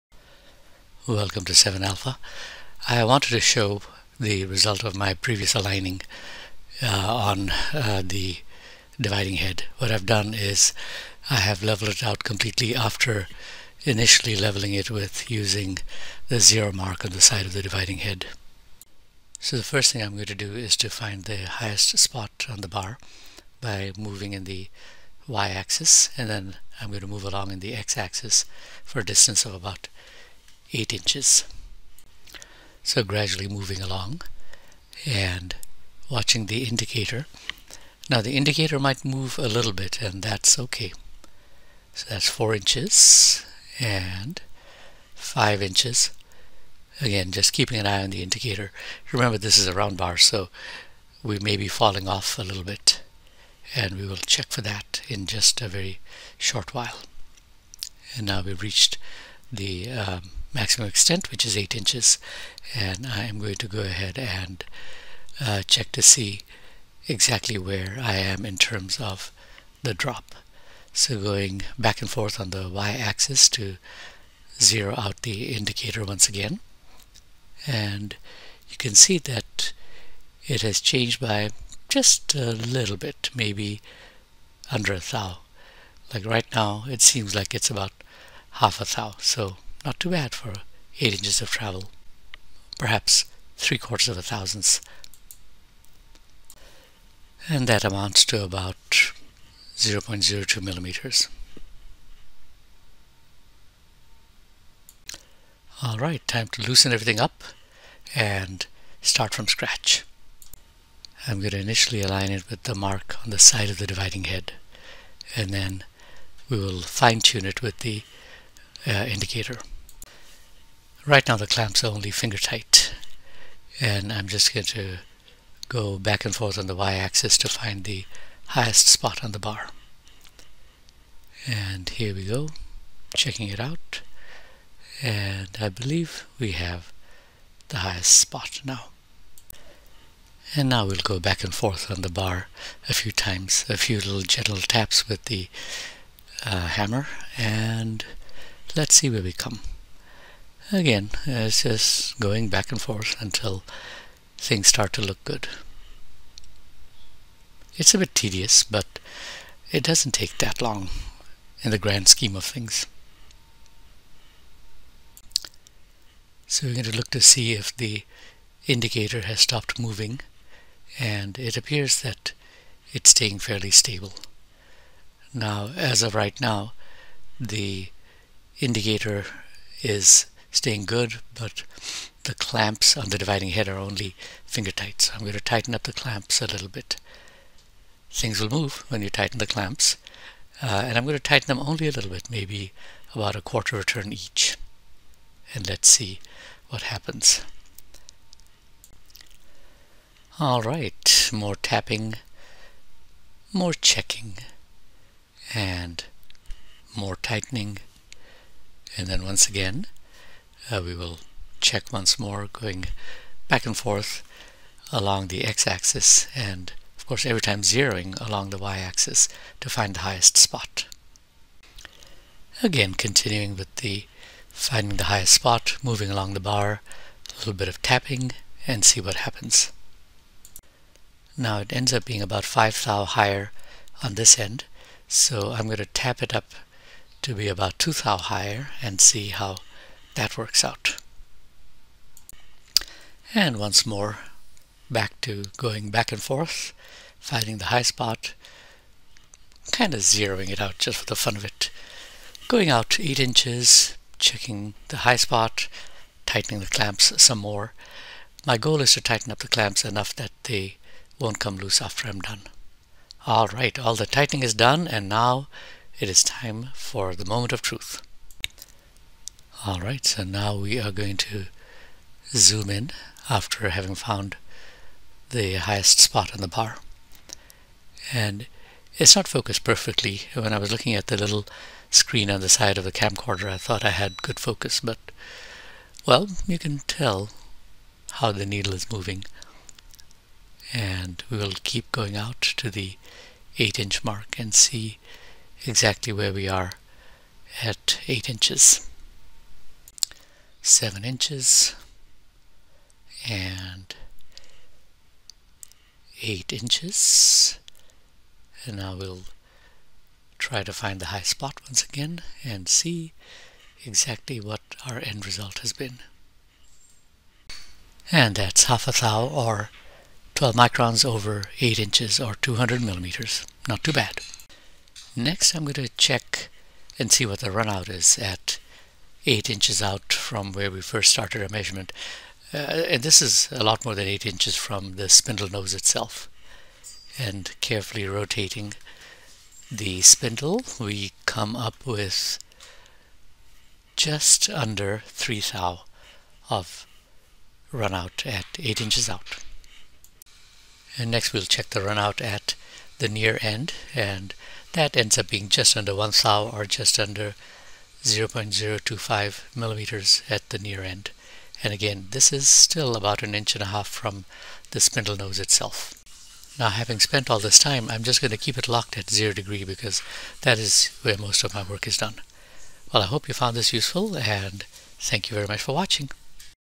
BS-1 Dividing head Horizontal Alignment
Set up the spindle of a dividing head in the horizontal plane accurately. More accurate than using the degree markings on the side of the head.
This is a BS-1 dividing head and supposedly has a Brown & Sharpe #7 taper. But it is a clone, and hardly anyone makes B&S tapers. I have found that a Morse 3 taper fits perfectly and I suspect that was what was used in the spindle.
I used an MT3 alignment bar, which is about $30-40. There is no reason why one could not do the same technique with a bar held in the chuck, though with the usual caveats that removing the bar from a 3-jaw chuck and reinstalling it may change the results.